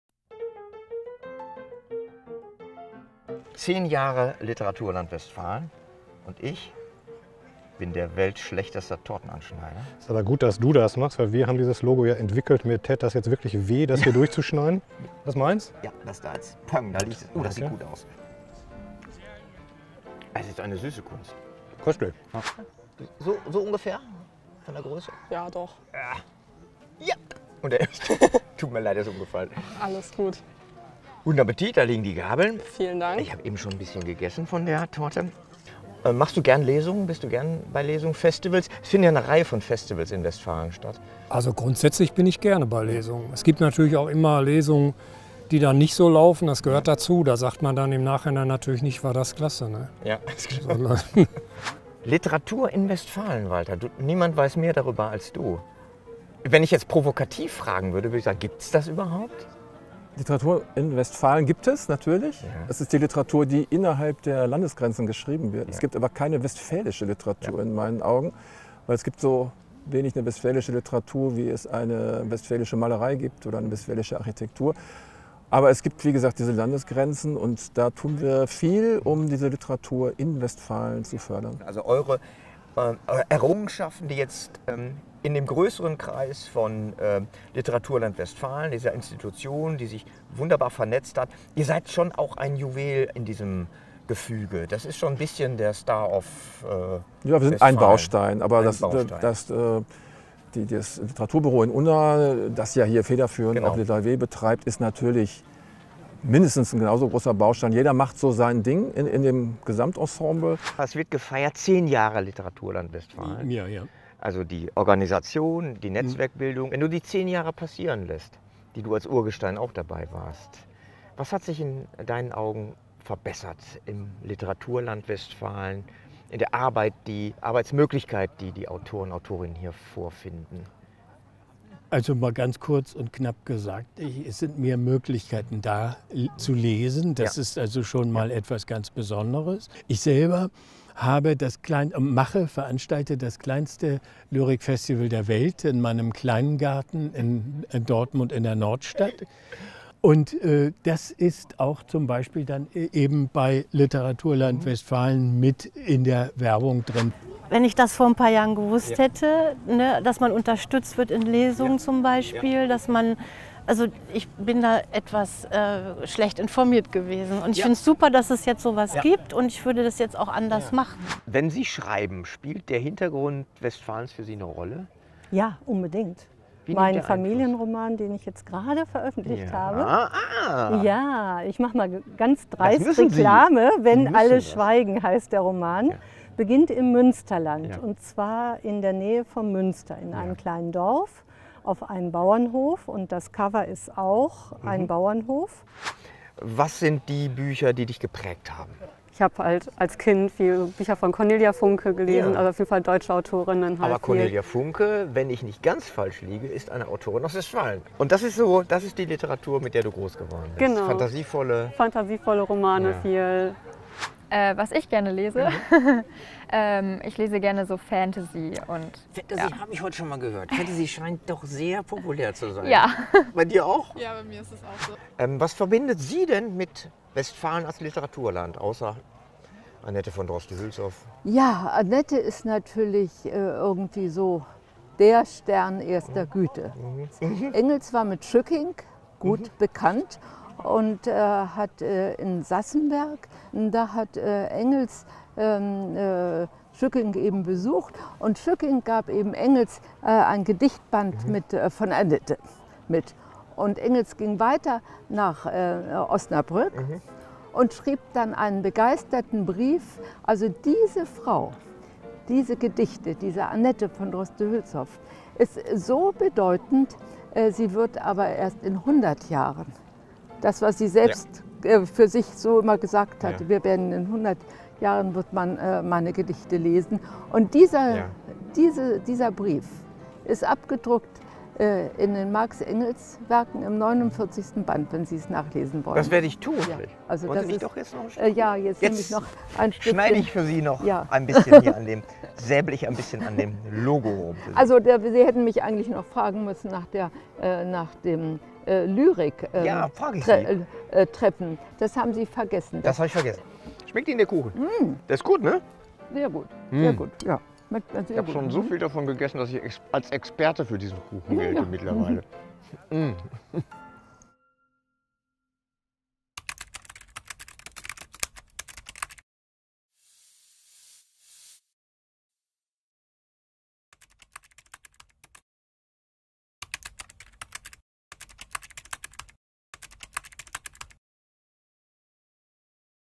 [lilaläum]-Aktion mit Thomas Krüger
Es gibt Torte! Zu Feier unseres [lilaläums] laden wir an verschiedenen Literaturorten in Westfalen gemeinsam mit Autor:innen aus der Region auf ein Stück lila Geburtstagstorte ein. In der schönen Atmosphäre des jährlichen Büchermarktes auf dem Kulturgut Haus Nottbeck in Oelde plauderte der aus Löhne stammende Autor Thomas Krüger am 11. September mit Gästen, Autor:innen und Veranstaltern. Die Highlights könnt ihr nun in diesem Video sehen.

Unter den Gesprächspartner:innen befanden sich unter anderem Autor:innen wie Ralf Thenior oder Cornelia Ertmer, der Leiter vom Kulturgut Haus Nottbeck – Museum für Westfälische Literatur, Walter Gödden, [lila we:]-Projektleiter Heiner Remmert sowie Besucherinnen und Ausstellerinnen des Büchermarktes. Dabei kamen unterhaltsame Einblicke und Perspektiven zusammen, die sich rund um Literatur, Lieblingsbücher und das literaturland westfalen drehen.

Weiter geht unsere Geburtstagsaktion am 10. Oktober in Arnsberg. In der Stadtbibliothek Neheim lädt die Krimiautorin und Kabarettistin Kathrin Heinrichs vor einer Lesung mit dem österreichischen Thrillerautor Andreas Gruber auf ein Stück lila Geburtstagstorte ein.

Kamera & Schnitt: Philipp Wachowitz